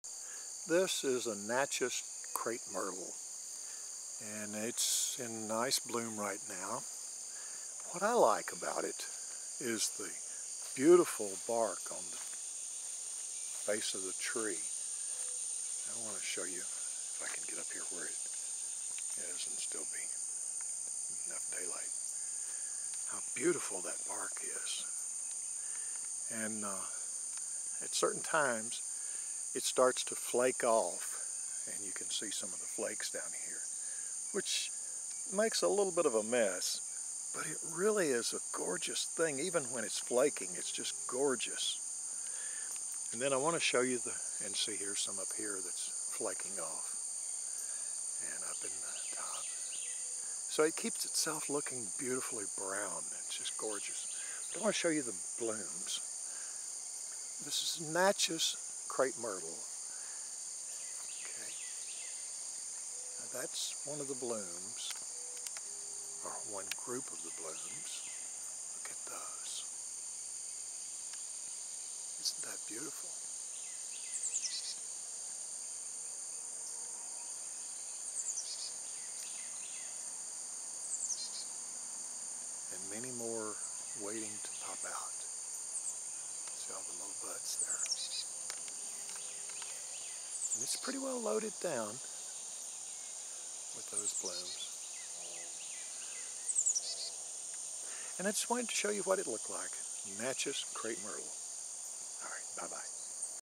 Natchez White Crepe Myrtle
This Natchez White Crepe Myrtle tree is blooming very nicely. Close ups of the blooms.